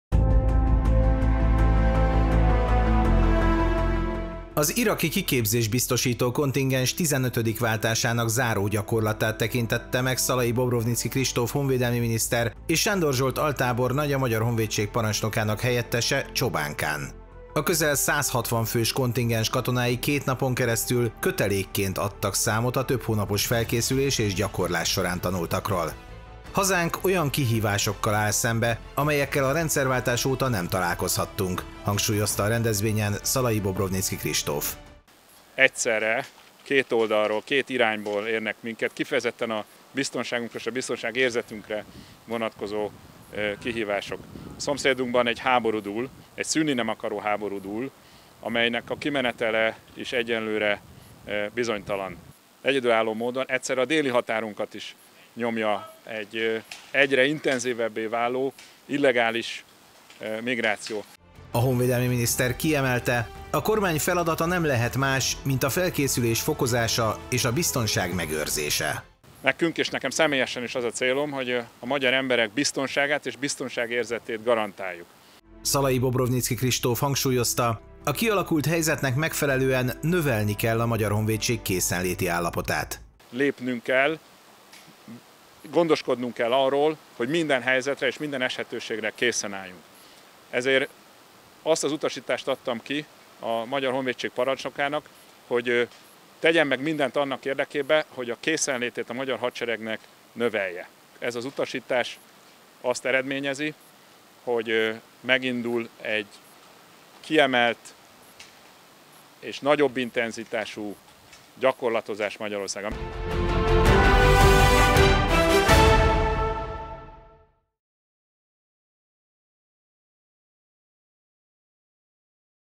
Szükség van a honvédség készenléti fokozatának emelésére
Hazánkat kettős, Keletről és Délről jelentkező biztonsági nyomás éri, ezért szükség van a Magyar Honvédség készenléti fokozatának emelésére - jelentette ki Szalay-Bobrovniczky Kristóf honvédelmi miniszter a csobánkai kiképzőbázison tartott sajtótájékoztatóján, július 13-án, szerdán.

Készítette: Blénesi Csaba, Ferenczi János, Hanula Gábor, Szűcs László, Tanka Balázs, Trautmann Balázs